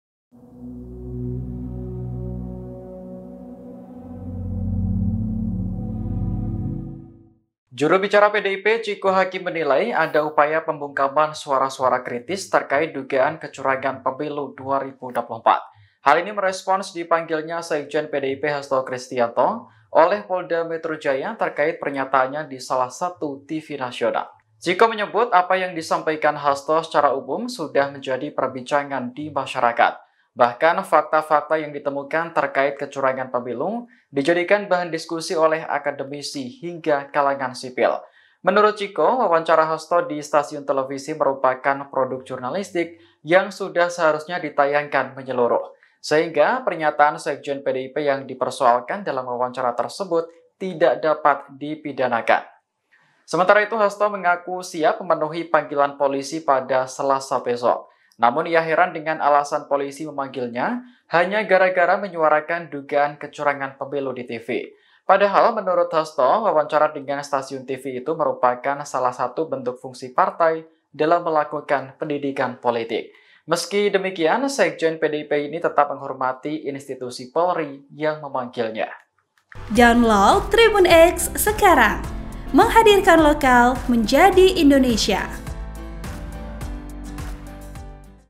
Respons PDIP soal Hasto Dipanggil Polisi seusai Wawancara di TV: Ini Pembungkaman Suara Kritis
TRIBUN-VIDEO.COM - Juru Bicara PDIP Chico Hakim menilai, ada upaya pembungkaman suara-suara kritis terkait dugaan kecurangan Pemilu 2024.

Hal ini merespons dipanggilnya Sekjen PDIP Hasto Kristiyanto oleh Polda Metro Jaya terkait pernyataannya di salah satu TV nasional.

Program: Tribunnews Update
Host: Agung Laksono
Editor Video: Muhammad Ulung Dzikrillah
Uploader: Panji Anggoro Putro